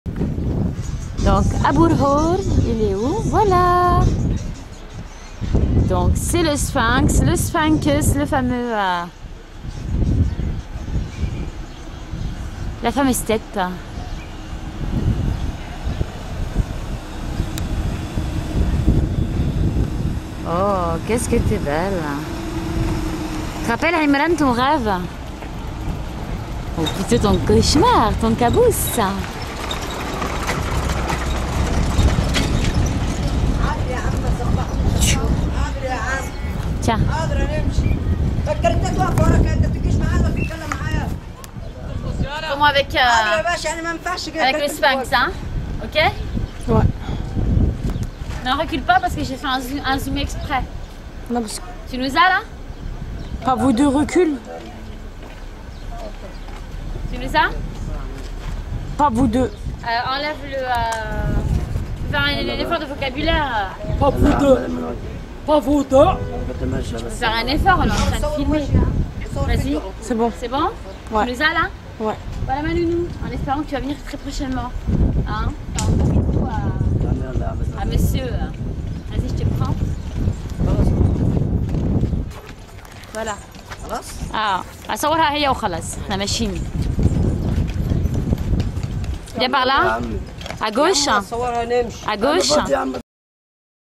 CAIRO LE SPHINX ABOU-EL- HOL // NORA TON VOCABULAIRE ... ME DESOLE!! h h h
C'est le père de la terreur qui veille toujours sur la nécropole de Gizéh, les yeux fixés sur le levant, un sourire énigmatique et presque ironique sur les lèvres.
ABOU-EL-HOL n'a jamais dévoilé le mystère de sa construction, unique au monde on ne compte plus le nombre de légendes qui l'entourent.
Ce sont les grecs qui l'appellent " sphinx " sa forme de lion était identique au monstre  de leur mythologie, un lion à tête de femme qui dévorait les voyageurs.
Sauve qui peut la vie .